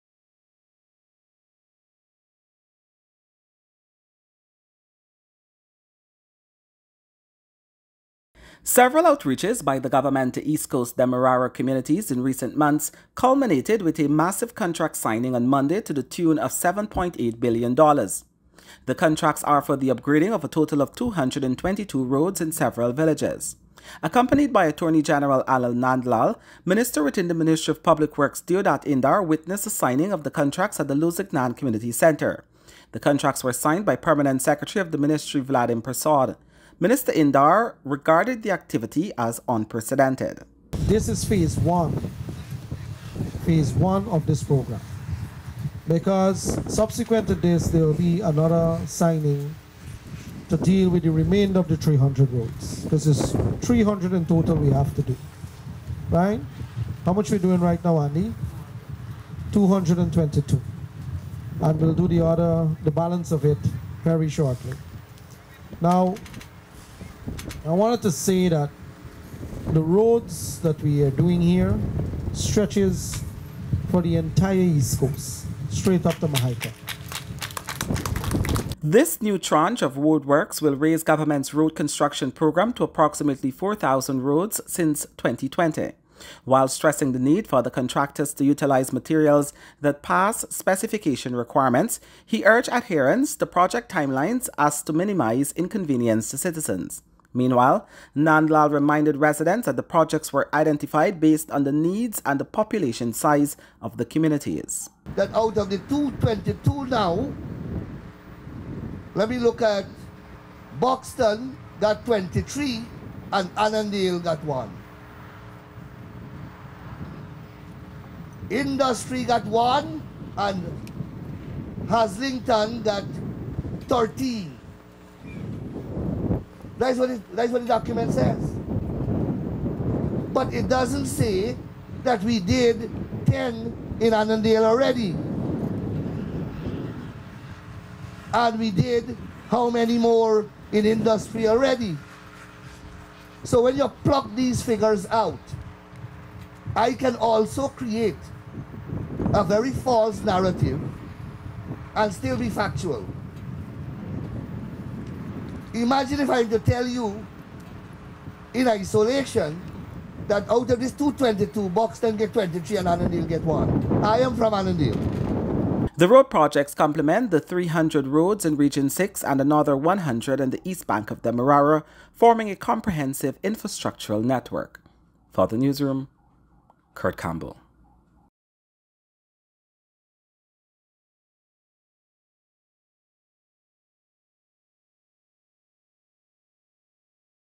222 E C D ROADS TO UNDERGO UPGRADES IN COMING MONTHS
The government says its move to upgrade a total of 300 roads along the East Coast of Demerara is unprecedented and groundbreaking but a promise it intends to deliver on with contracts already signed for well over 200 of those roads on Monday.